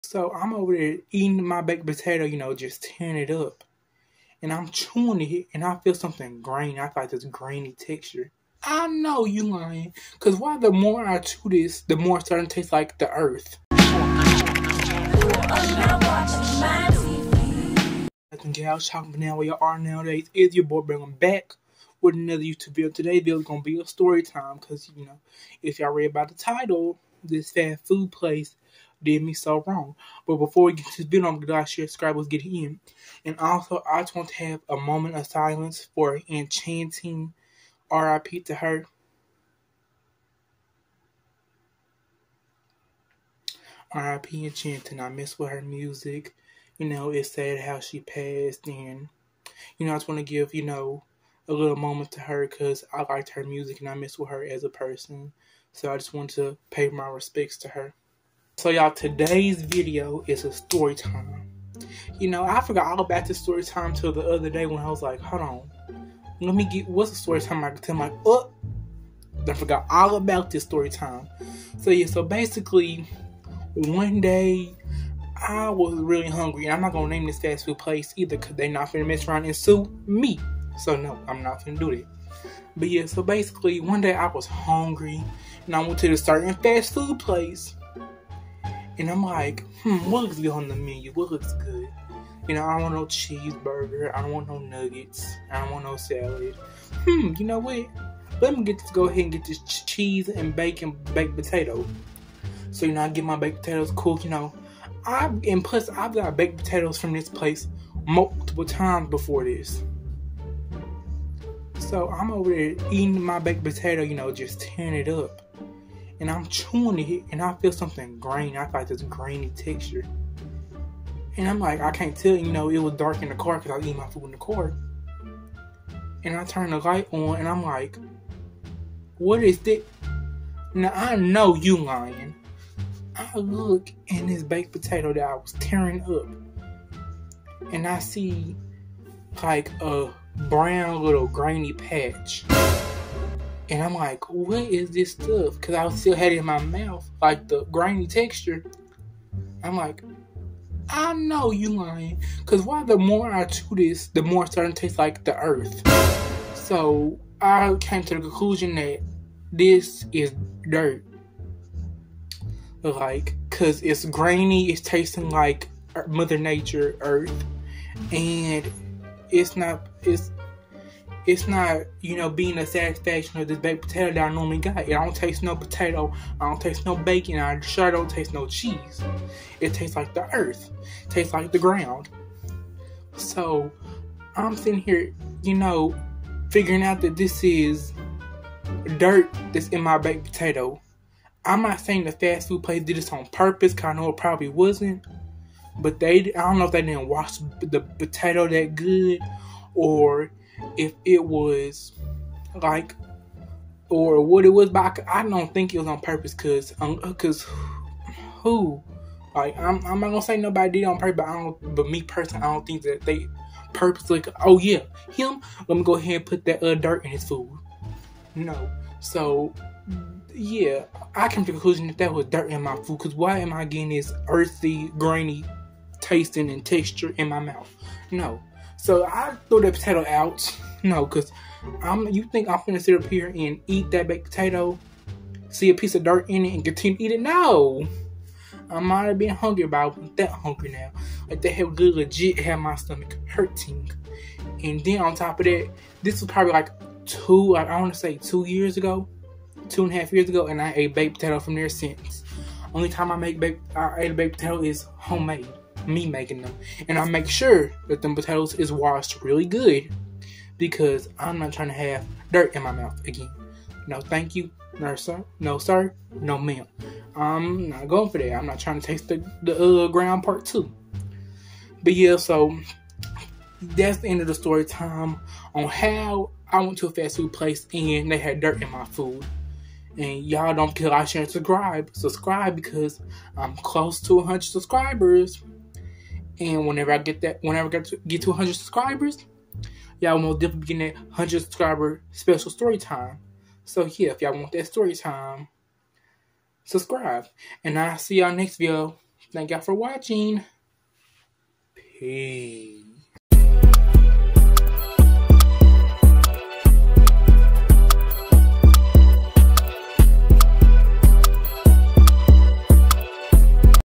FAST FOOD PLACE BUT DIRT IN MY FOOD|STORYTIME
Today's video is a storytime about a time where I went to a fast food place and as I was eating my food I found a patch of dirt in my food. Let me know if you have ever had anything in your food. Hoped y'all enjoyed today's video.